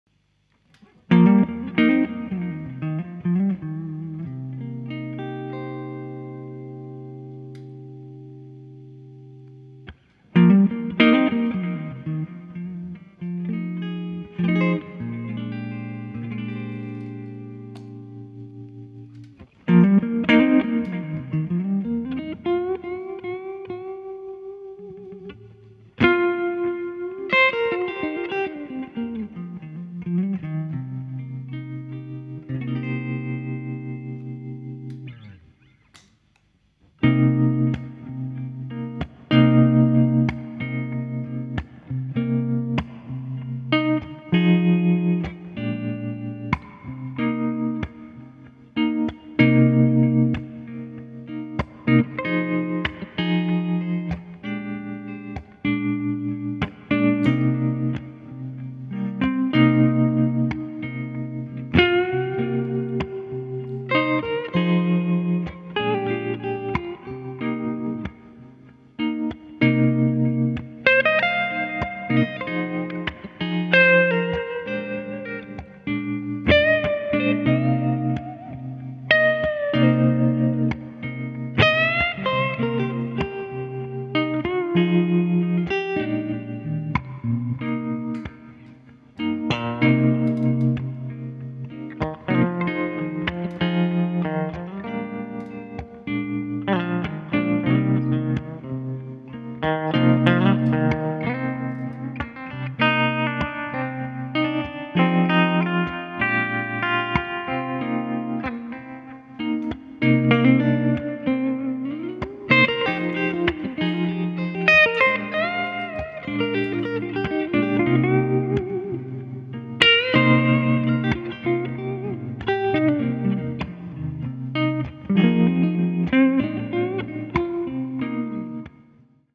2004 Gibson Les Paul Tom Murphy aged '59 Reissue Part1
Boss Looper RC-2 Clean only, Amp: Fender Vibroverb (original 1963), Vahlbruch SpaceTime Delay,